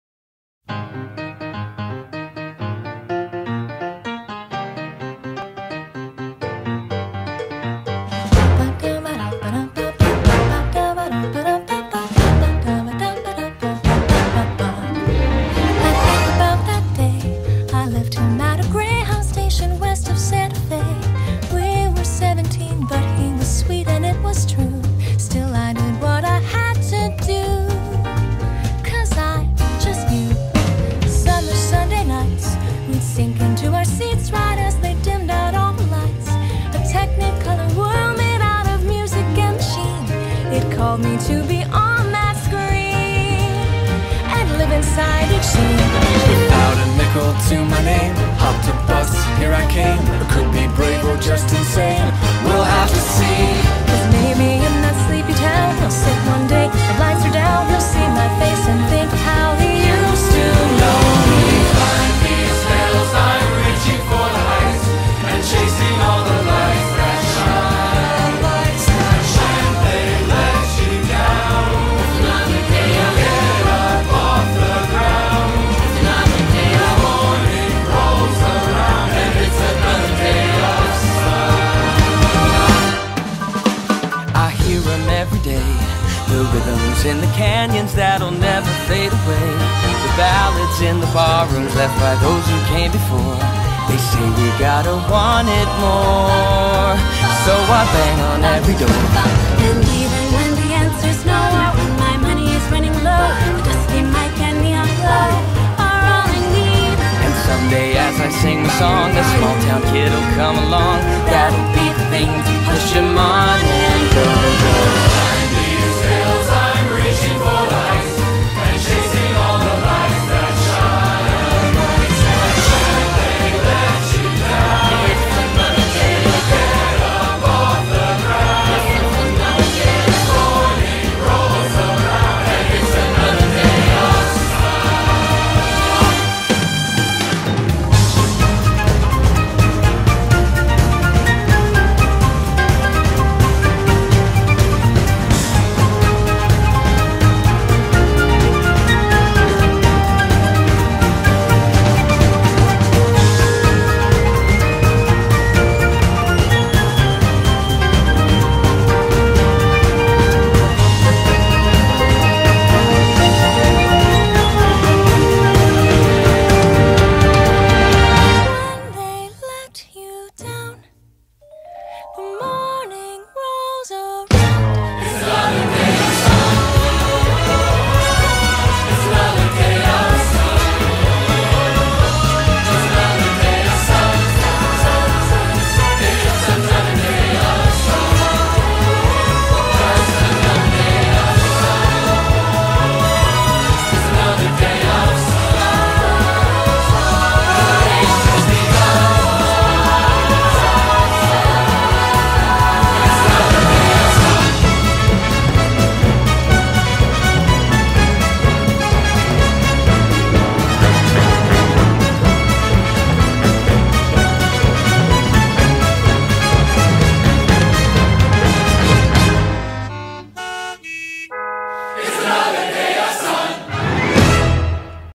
Moore Family Vacation 2018
Here’s a short video summarizing our amazing (and humid) trip to New Orleans